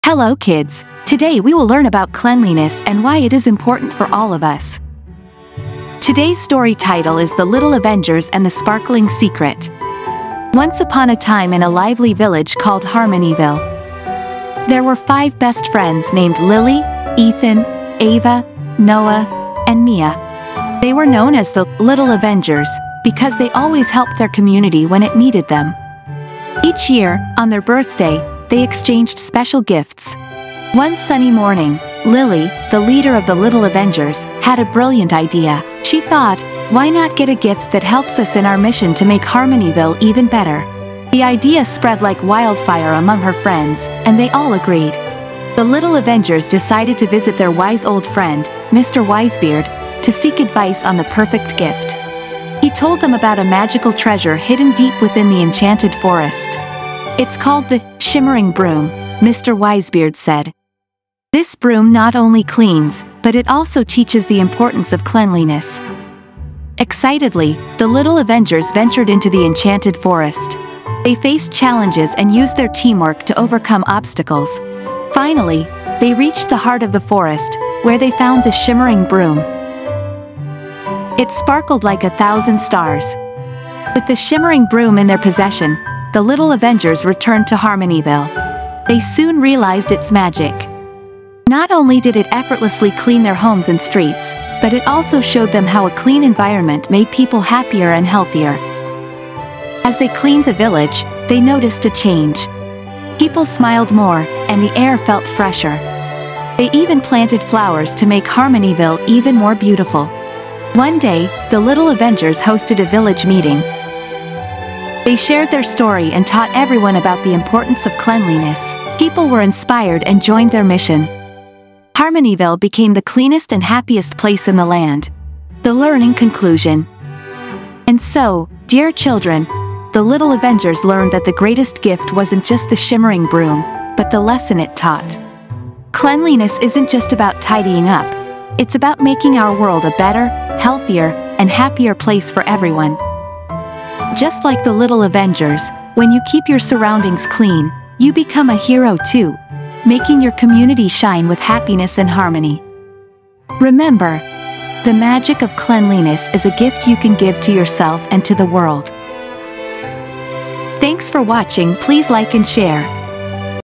Superhero Motivational video for kids learning | Little Marvels
Title: The Little Avengers and the Sparkling Secret

Once upon a time in a lively village called Harmonyville, there were five best friends named Lily, Ethan, Ava, Noah, and Mia. They were known as the "Little Avengers" because they always helped their community when it needed them. Each year, on their birthday, they exchanged special gifts.
One sunny morning, Lily, the leader of the Little Avengers, had a brilliant idea. She thought, "Why not get a gift that helps us in our mission to make Harmonyville even better?" The idea spread like wildfire among her friends, and they all agreed.
The Little Avengers decided to visit their wise old friend, Mr. Wisebeard, to seek advice on the perfect gift. He told them about a magical treasure hidden deep within the Enchanted Forest. "It's called the 'Shimmering Broom,'" Mr. Wisebeard said. "This broom not only cleans, but it also teaches the importance of cleanliness."
Excitedly, the Little Avengers ventured into the Enchanted Forest. They faced challenges and used their teamwork to overcome obstacles. Finally, they reached the heart of the forest, where they found the Shimmering Broom. It sparkled like a thousand stars.
With the Shimmering Broom in their possession, the Little Avengers returned to Harmonyville. They soon realized its magic. Not only did it effortlessly clean their homes and streets, but it also showed them how a clean environment made people happier and healthier.
As they cleaned the village, they noticed a change. People smiled more, and the air felt fresher. They even planted flowers to make Harmonyville even more beautiful.
One day, the Little Avengers hosted a village meeting. They shared their story and taught everyone about the importance of cleanliness. People were inspired and joined their mission. Harmonyville became the cleanest and happiest place in the land.
The Learning Conclusion:
And so, dear children, the Little Avengers learned that the greatest gift wasn't just the Shimmering Broom, but the lesson it taught. Cleanliness isn't just about tidying up; it's about making our world a better, healthier, and happier place for everyone. Just like the Little Avengers, when you keep your surroundings clean, you become a hero too, making your community shine with happiness and harmony.
Remember, the magic of cleanliness is a gift you can give to yourself and to the world.